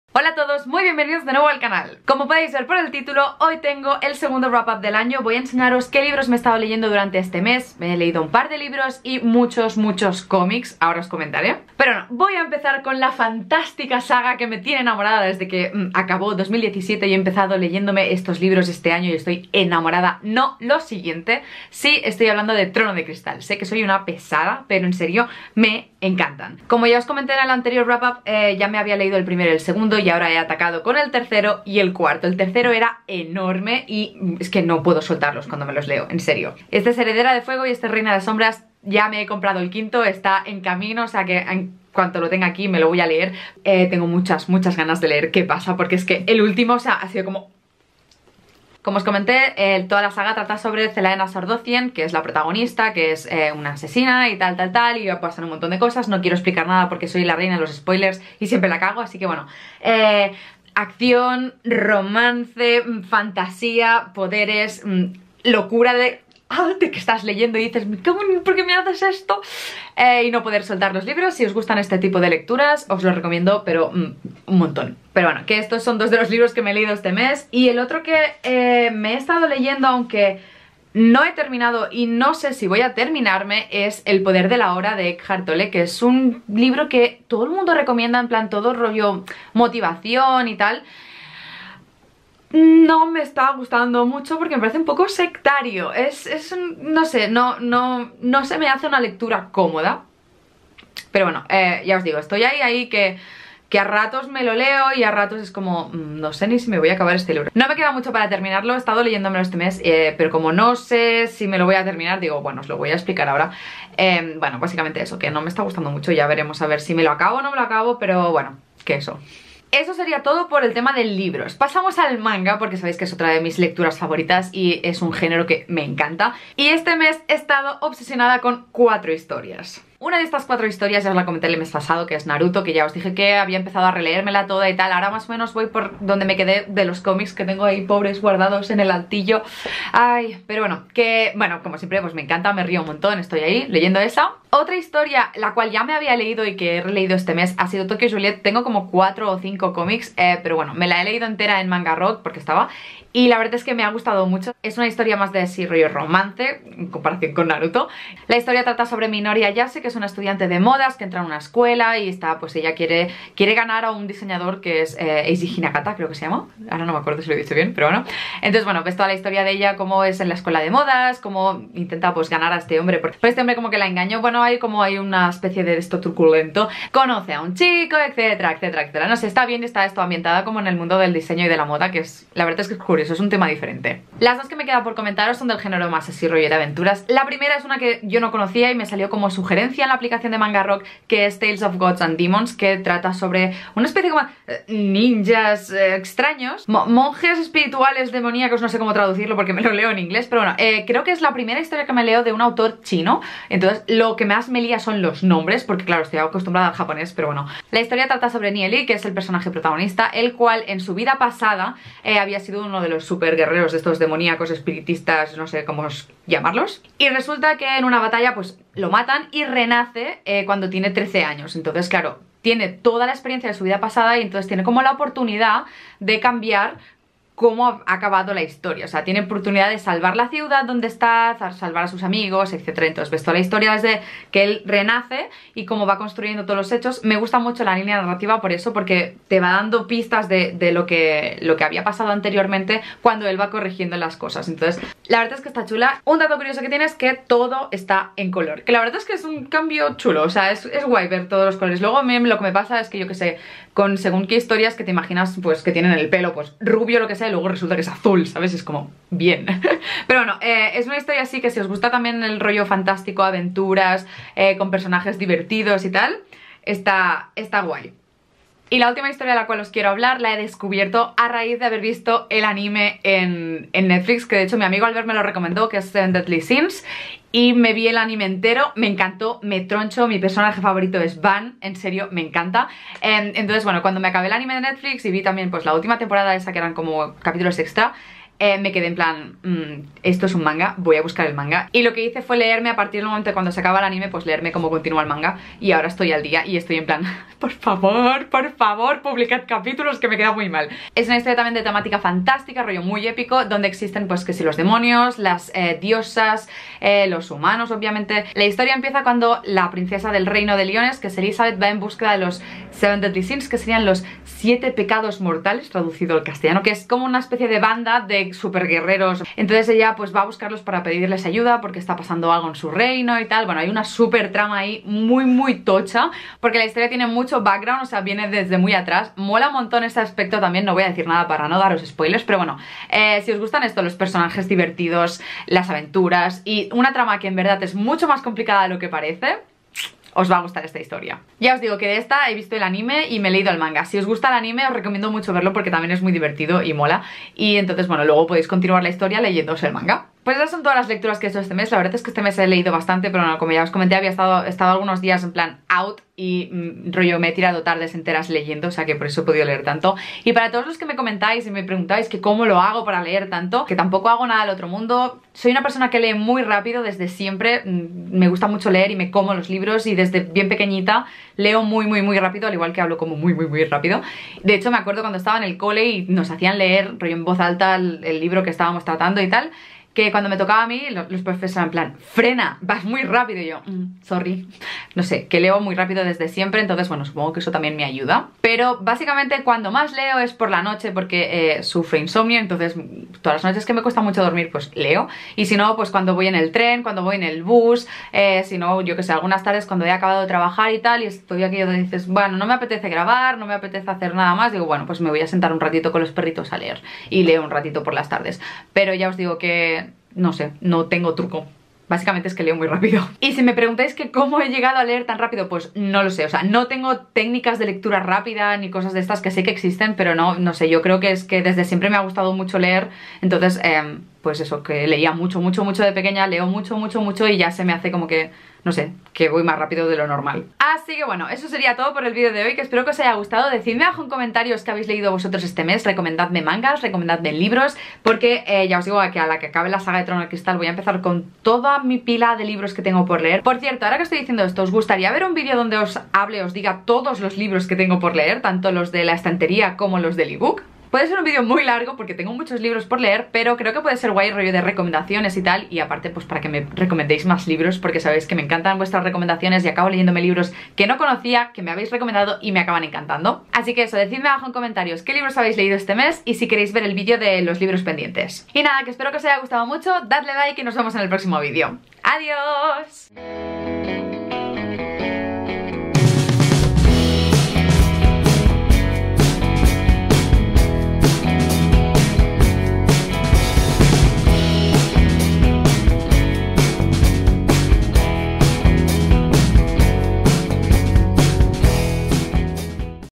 Mis últimas lecturas · WRAP UP FEBRERO 2018 | Christine Hug
Hola! En el vídeo de hoy os hablo de mis lecturas de febrero! :D

MI LIBRO · DIARIO BULLET:
Talavera (baldosas)

·Editado con Final Cut Pro X
·Grabado con: